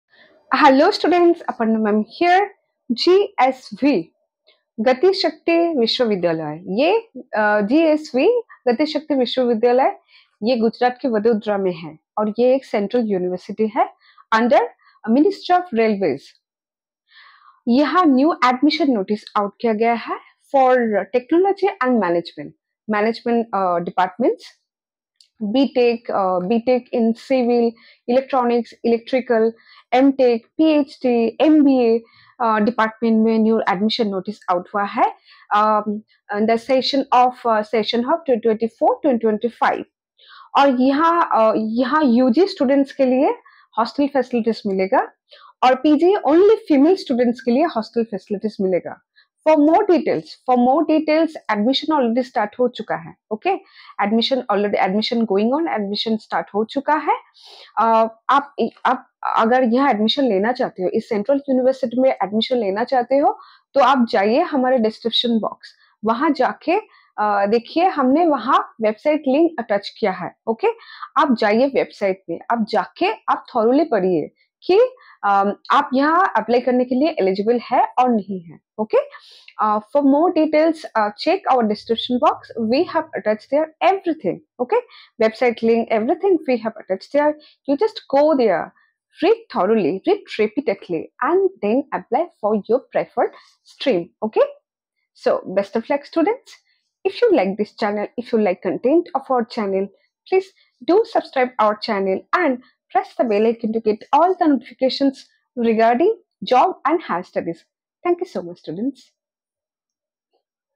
Gati Shakti VishwaVidyalaya Admission 2024 | NRTI Admission 2024
Hello Friends! I hope you all are doing great. Today's video is about a latest job Notification out from Gati Shakti VishwaVidyalaya, Vadodara Gujarat (Central University established by an act of Parliament vide Central universities (Ammendment) Act 2022. This University is sponsored by the Ministry of Railways, Government Of India) invites applications for Admission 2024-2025 to the following programmes (1) Engineering Under Graduate-B.Tech_Civil in Specialization in Rail/Electronics & Telecommunication _Spwcialization in Rail Engineering/EE/AI & Data Science _Specialized in transport and logistics/Aviation Engineering (2) Management -MBA-Logistics & Supply chain management/Ports and shipping (3) Post Graduate -M.Tech-Intelligent transport system/M.Tech-Railway Engineering (4) Management -MBA Supply chain management/MBA-Multi modal transportation/MBA- Metro Rail Management (4) PHD -Engineeing Management. So that we can bring more quality content for you.